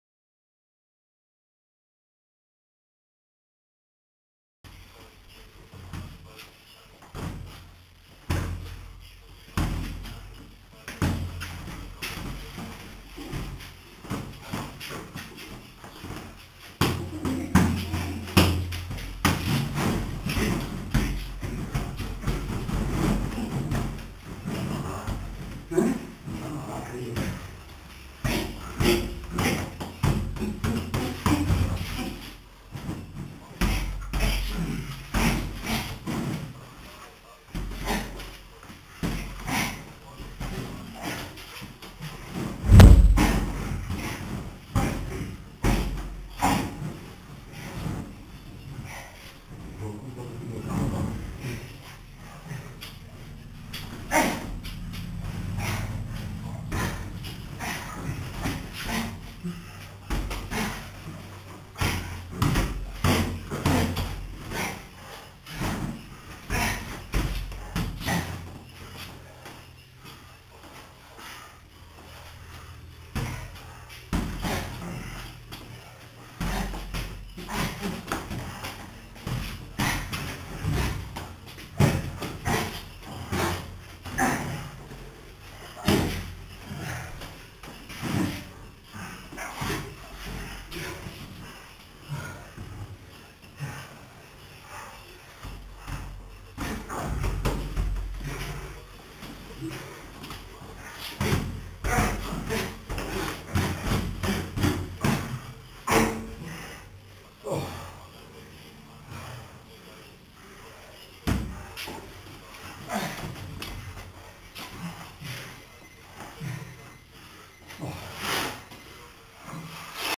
Runde07 --- Werner vs Heinrich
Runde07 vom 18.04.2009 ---
boxen Werner vs. Heinrich  ---
Anzahl Treffer:  -
Werner   26  ---
Heinrich 74 ---